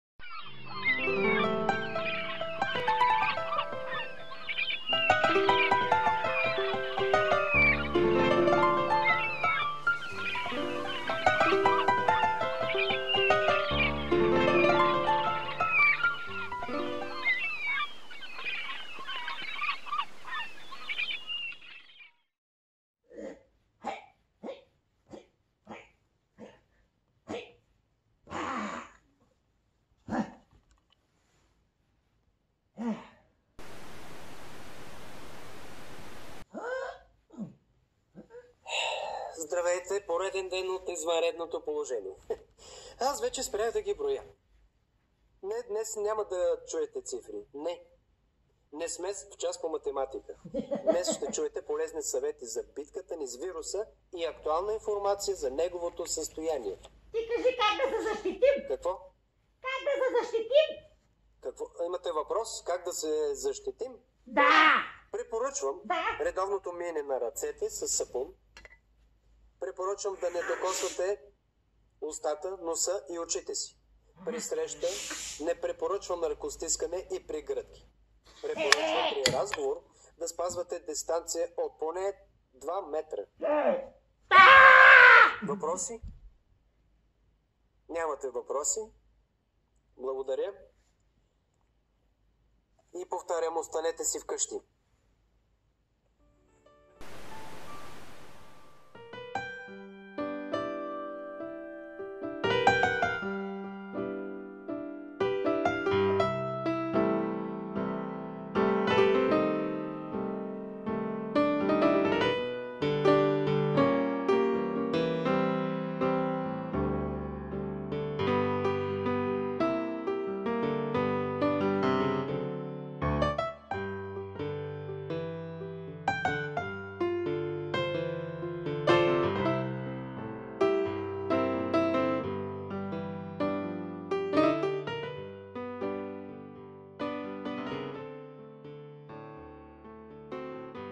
Куклен "кризисен щаб" и новини. еп1 Какво правят куклените герои по време на кризата.
Таралеж и заек.